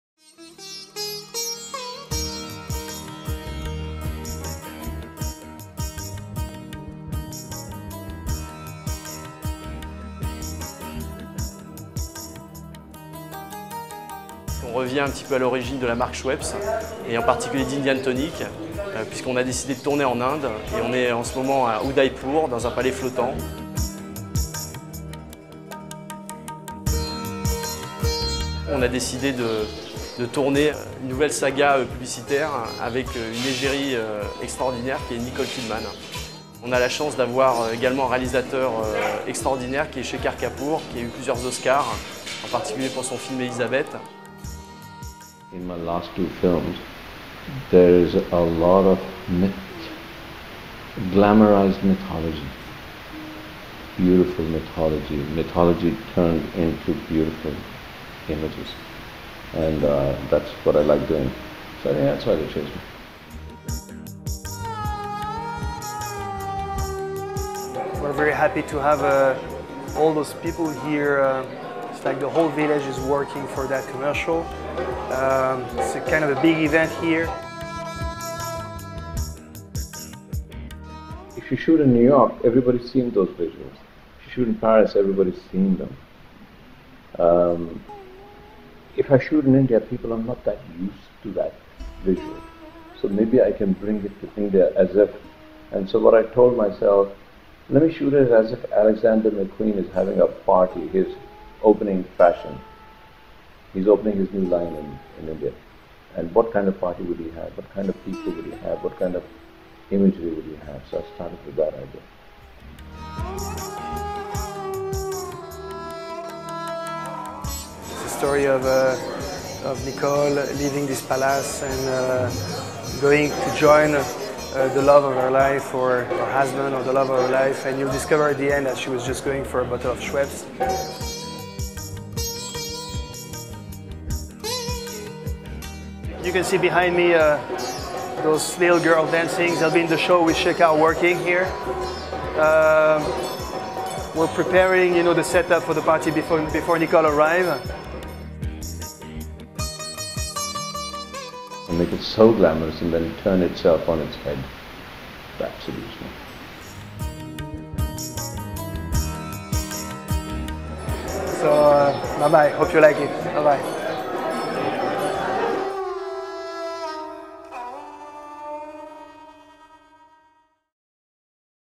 Schweppes - the making of
A dream setting, one of todays most glamorous icons, Nicole Kidman, and a celebrated directorfor its 2009 campaign, Schweppes is going to Hollywood. Indeed, the brands new advertising saga devised by FRED & FARID, PARIS would do the movie industry proud.
Shot in central India  where Indian Tonic first originated  the film sets the stage for a romantic love story with all the magic of a super-production, and a surprise twist at the end.

To play the heroine, Schweppes asked Academy Award winning actress Nicole Kidman to lend her talent and her beauty. A beauty captured by the Indian film director Shekhar Kapur, who won an Oscar for his film Elizabeth, the Golden Age.

On the set, nothing was left to chance when filming began last March. Starring opposite Nicole Kidman were two top names from Indias film industry: Arjun Rampal, a top Bollywood star, and Rubina Ali Qureshi, discovered in the worldwide blockbuster Slumdog Millionaire.

Lastly, this dream cast needed a décor to match and got it with an extravagant white marble palace floating on a lake at the heart of Rajasthan: the famous Lake Palace of Udaipur. Shekhar Kapur has created for Schweppes a sparkling romantic adventure at the heart of the sub-continent with a role tailor-made for an actress who is rarely where you expect her to be.

Sheer glamour. Action!